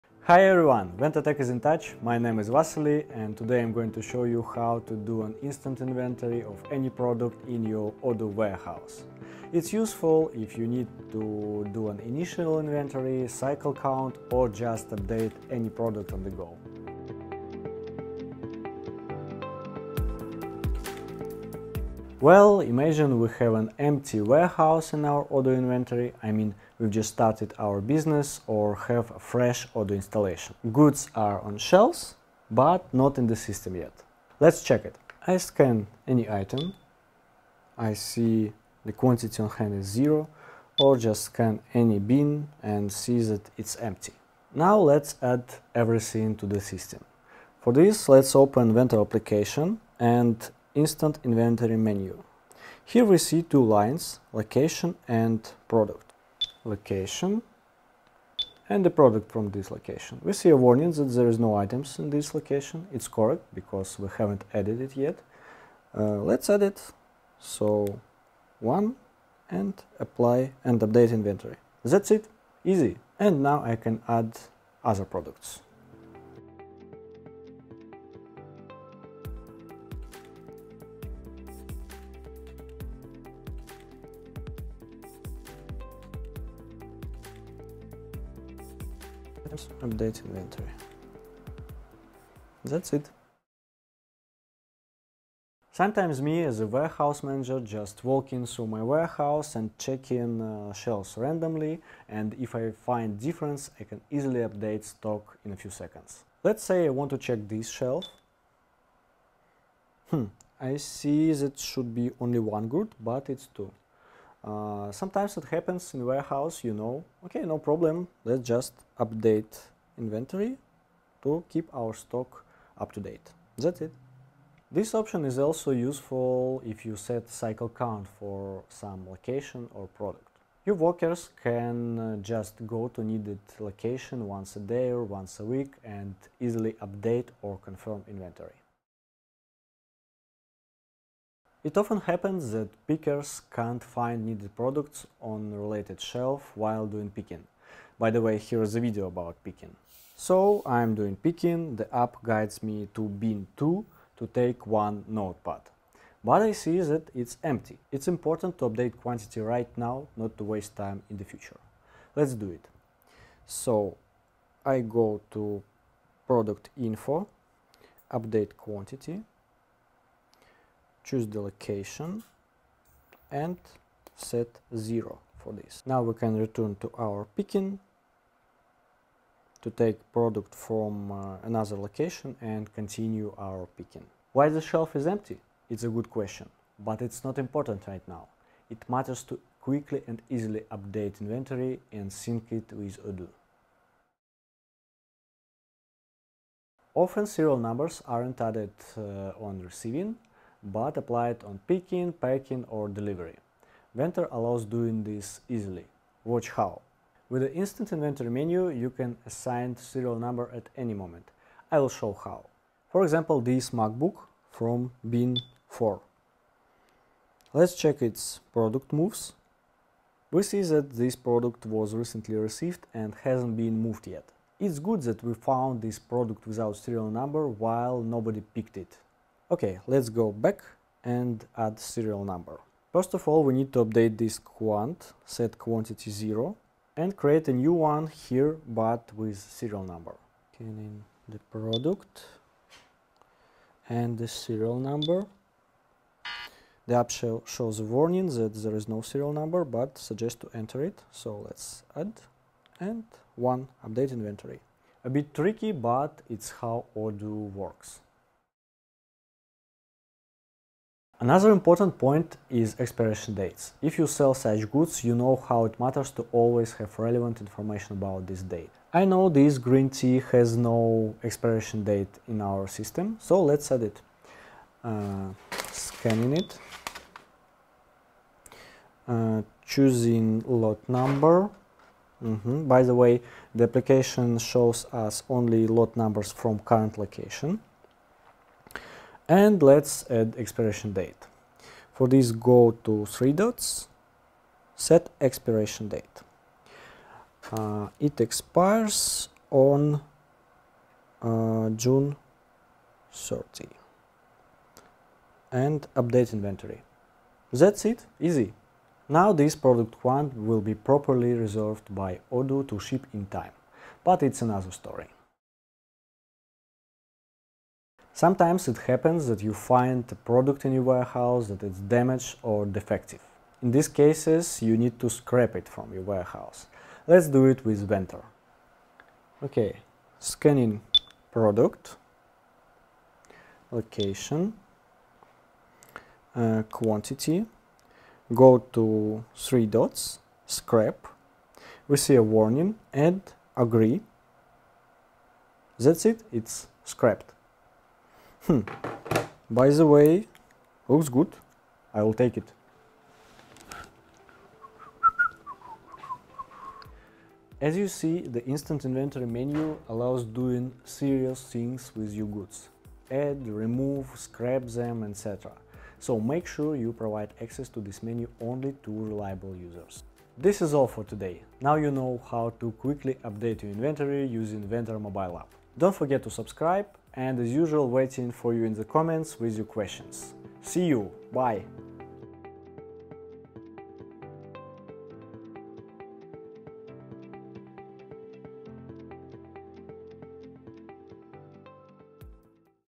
Make Odoo inventory quickly and accurately using Ventor app | VentorTech
Do you need to process an Odoo initial inventory, cycle count, or just update some qty on the go? This video is for you! You will see how to quickly update your Odoo inventory using the Ventor mobile app.

Timecodes:
00:21 Initial Odoo Inventory
1:33 Cycle count and Update qty on the go
2:18 Update Odoo inventory while doing other warehouse operations
3:14 Adding Odoo serial number
4:18 Adding Odoo expiration date
5:15 Odoo Scrap